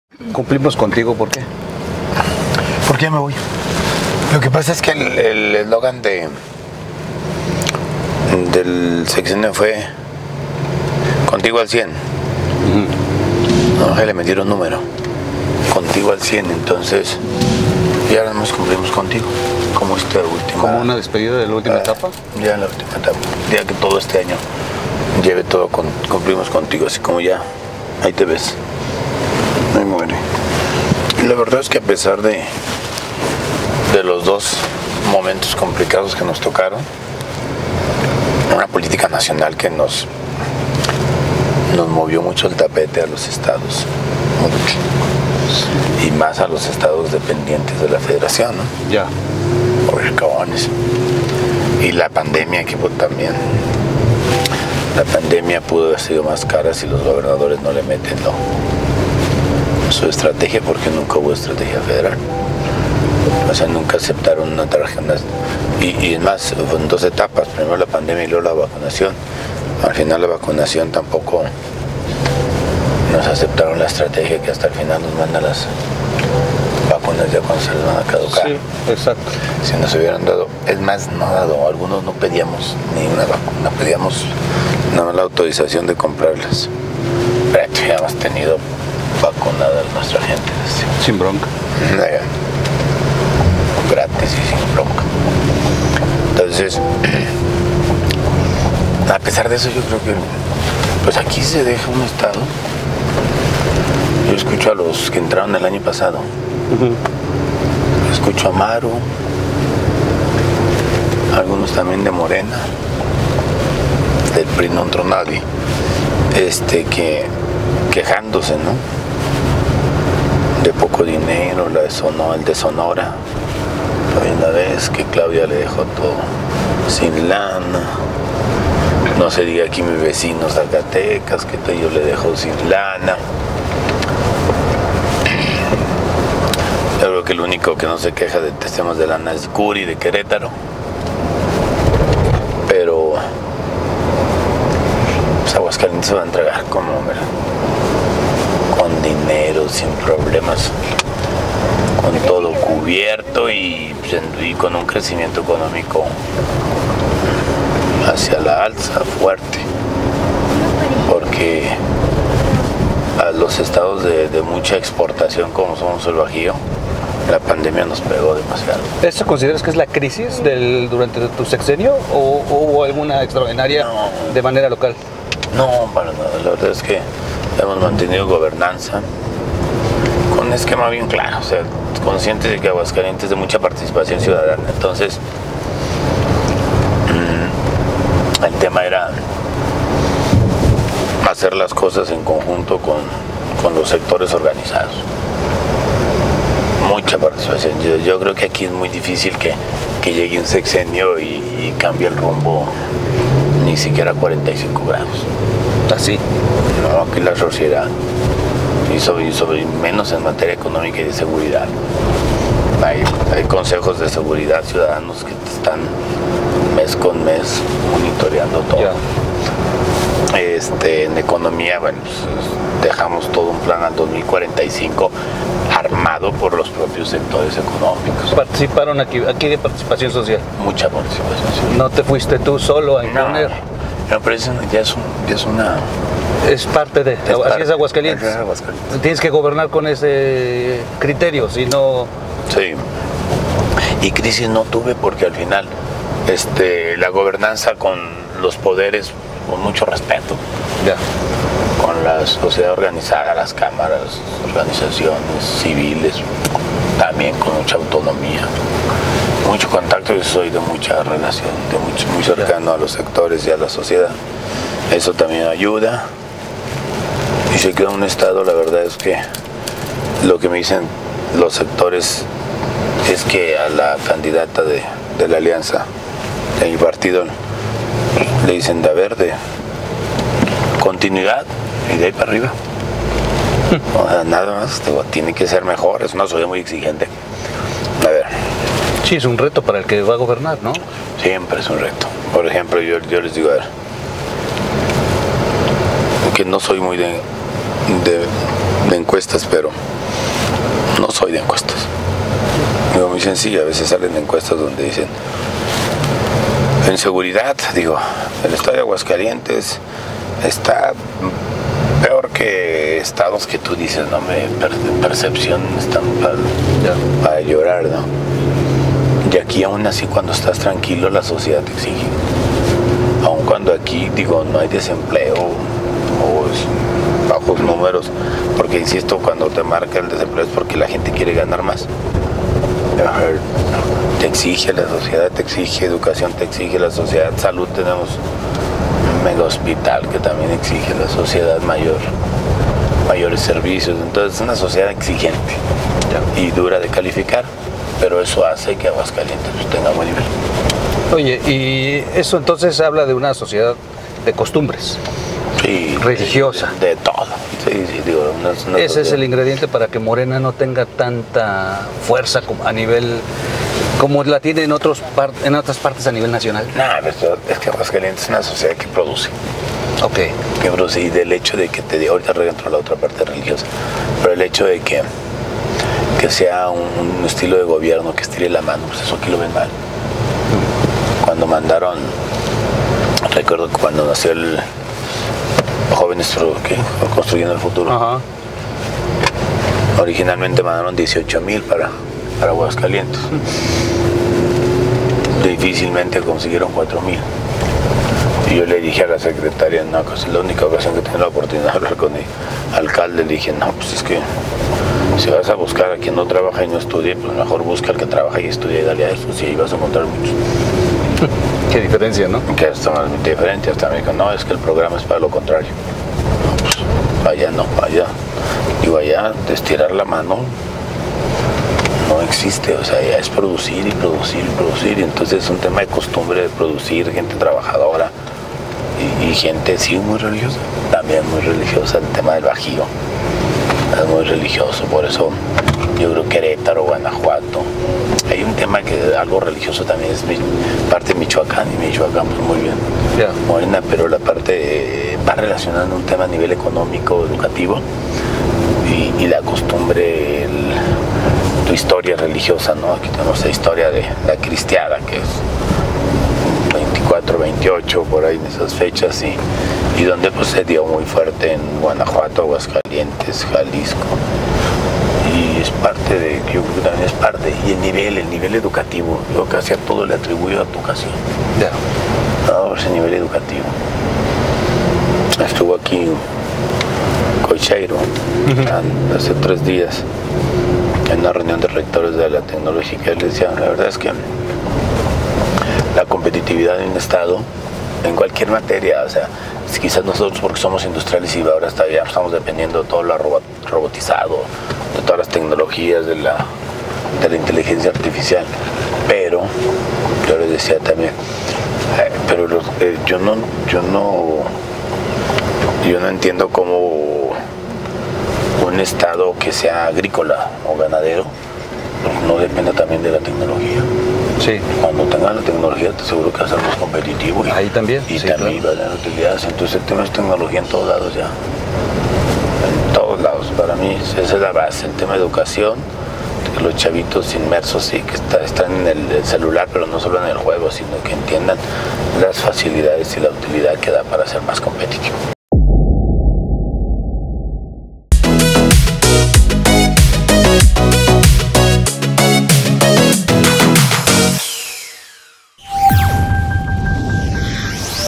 IMPERDIBLE! EL TROLL hace SUFRIR al GOBERNADOR de AGUASCALIENTES, Martín Orozco
4K EXCLUSIVO!!: Martín Orozco nos da una gran lección del como Gobernar. Así es como Aguascalientes ha logrado ser uno de los Estados con más desarrollo económica del País.

►Amazon Music